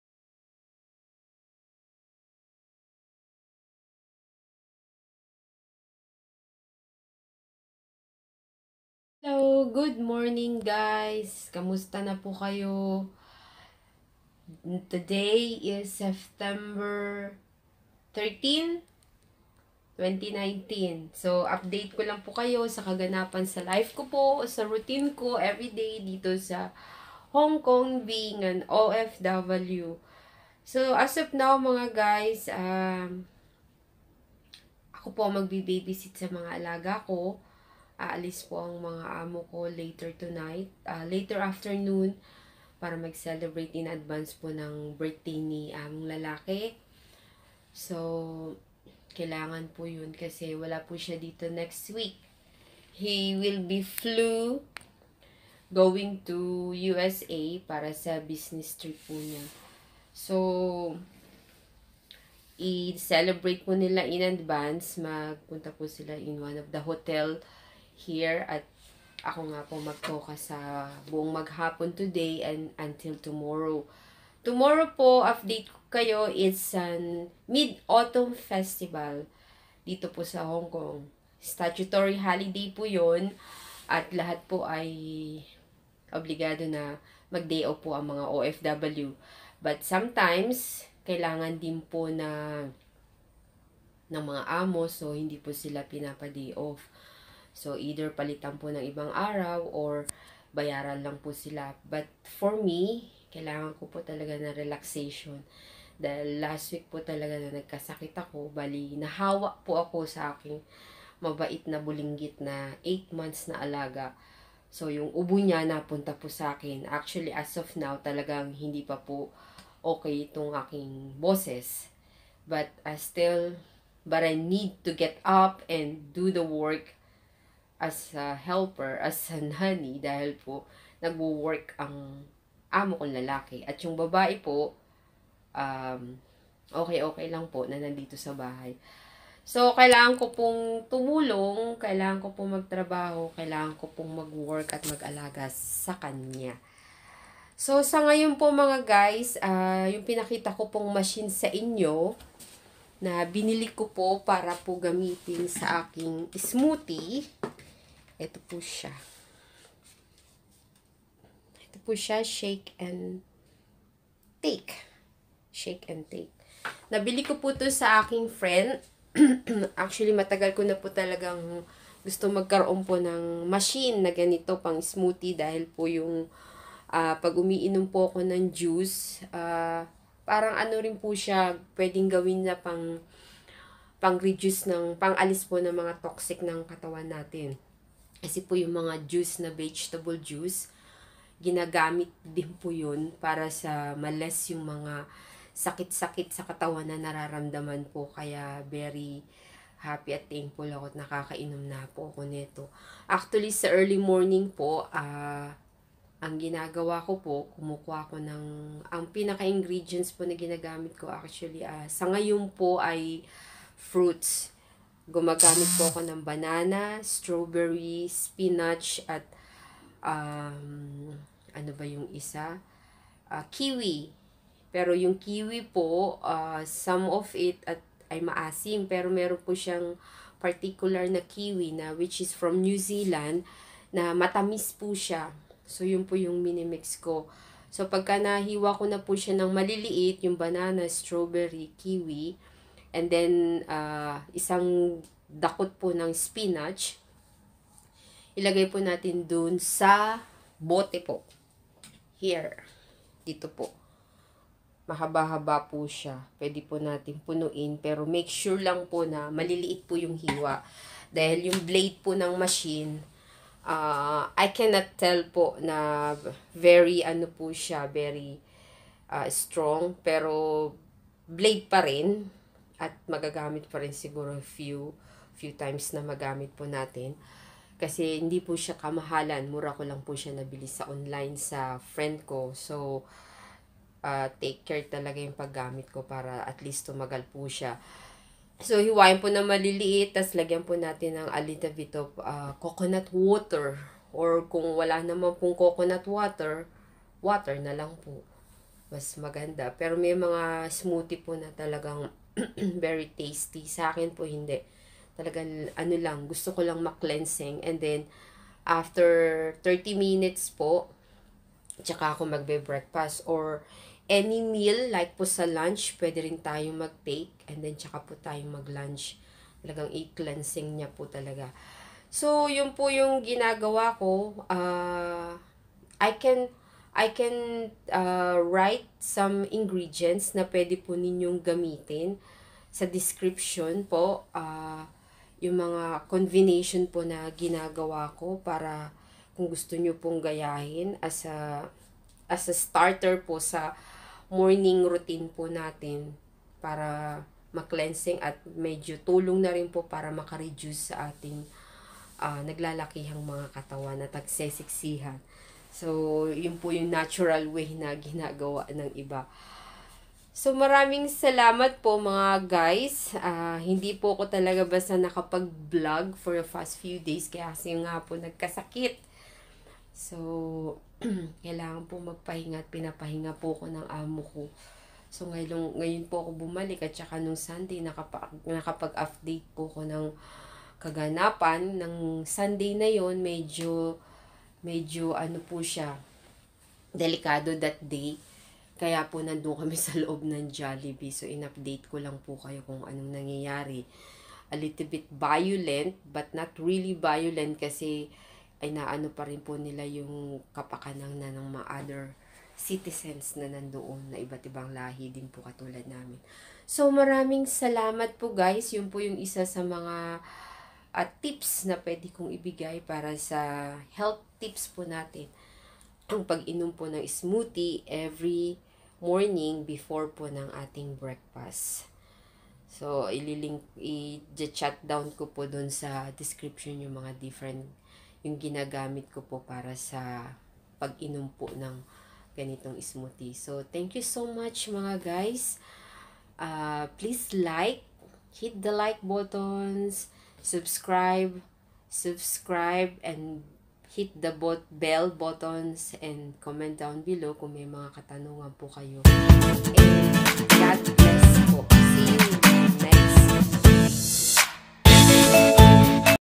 The affordable machine i used for my smoothie
Here the ingredients I used
half banana
6 pieces strawberies
1 kiwi
a handful of spinach
1 c of coconut water
Drink 30 mins before breakfast 😉😉